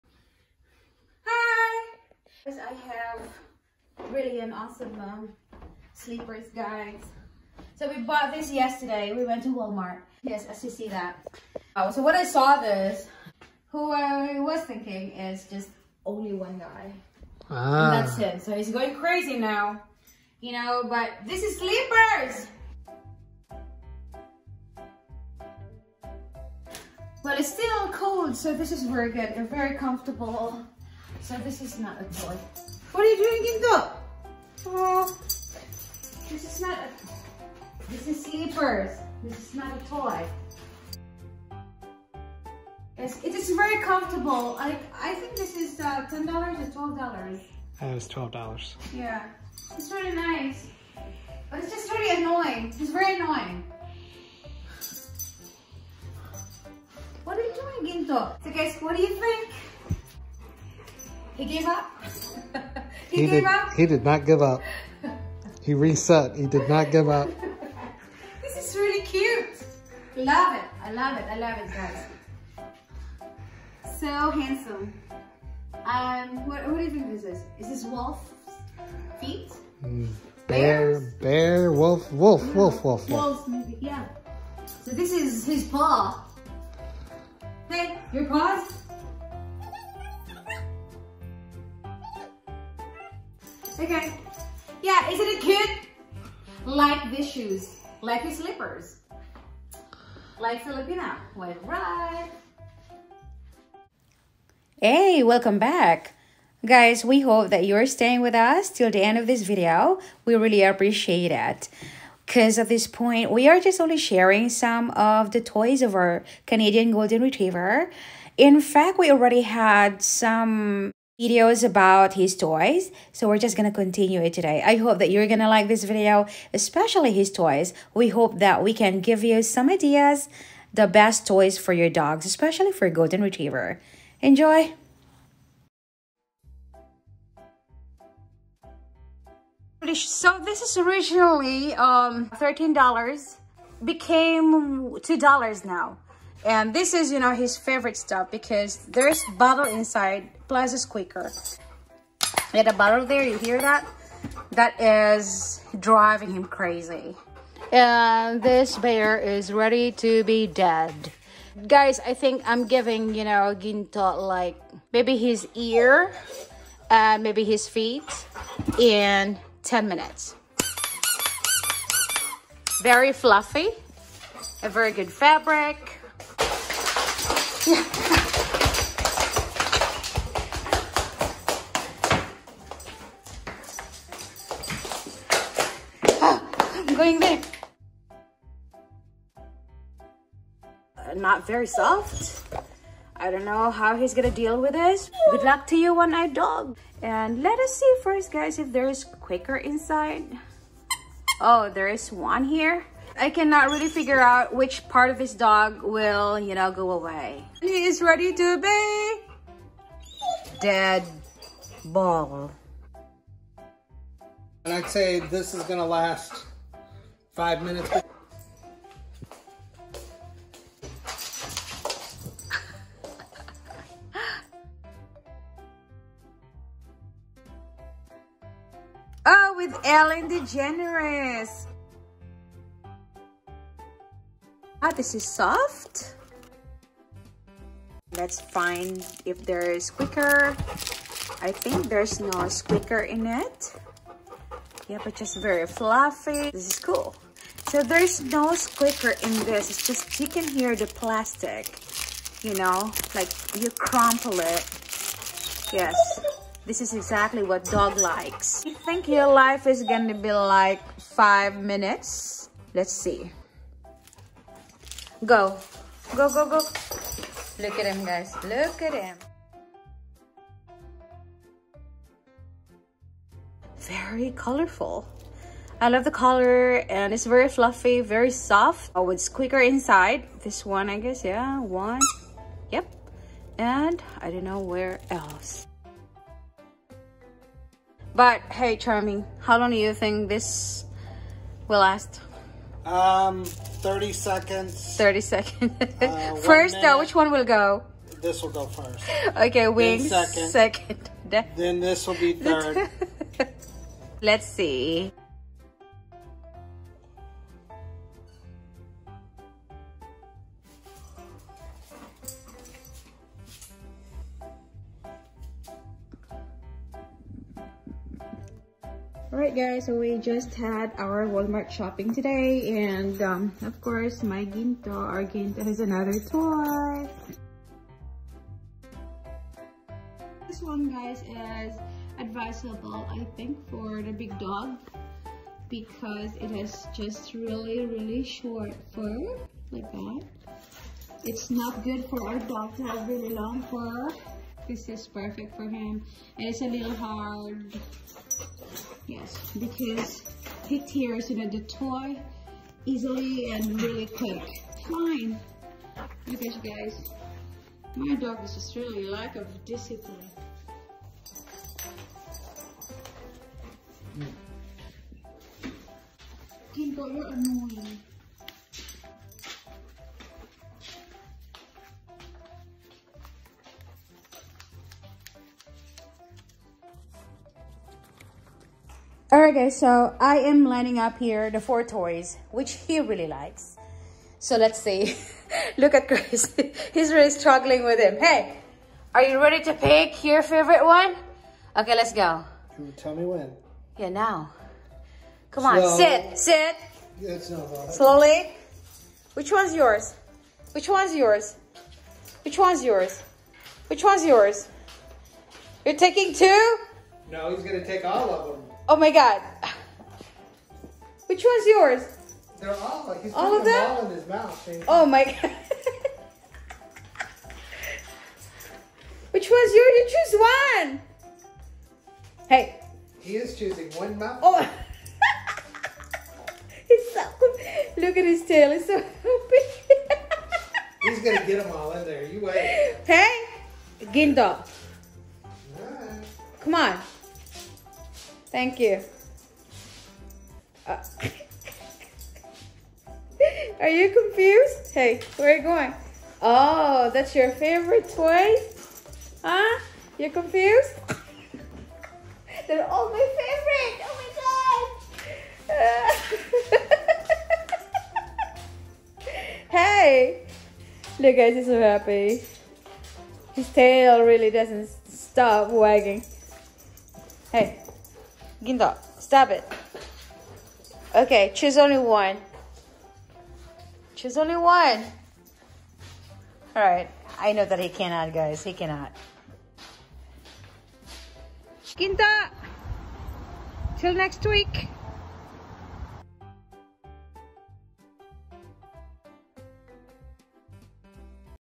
Golden Retriever Hilarious Adorable Reaction to my Paw Slippers
Been a while I didn't share Ginto's toys with you guys, so I hope you will enjoy this video as we want to share some good and ideal toys  for a golden retriever with you all!
We truly appreciate  your time watching this start to end.